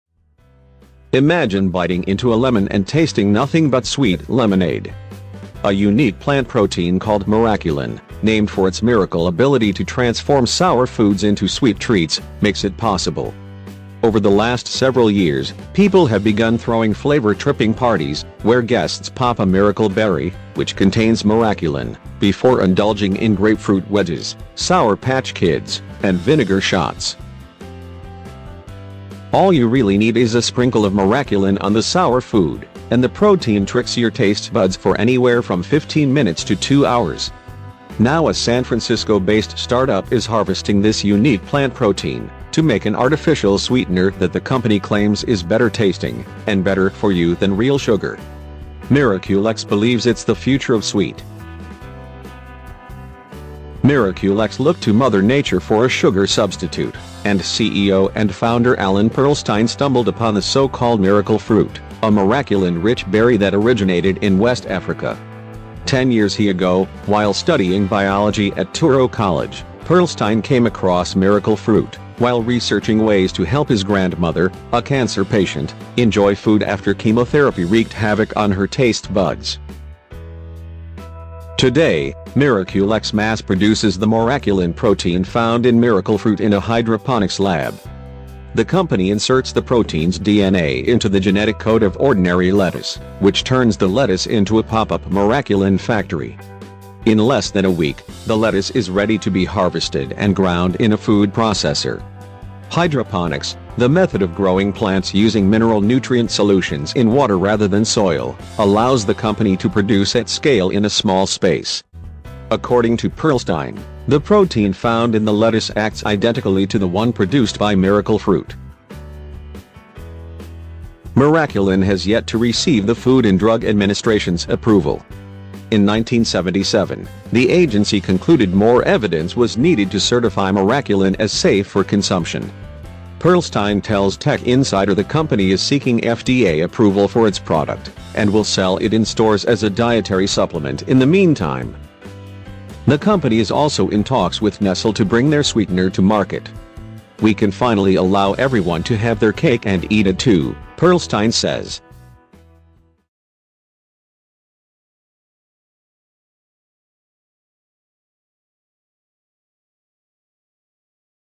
This sweetener uses a miracle protein to turn sour foods sweet
Each serving costs $0.32 to make, though the company expects that price to drop in the coming years.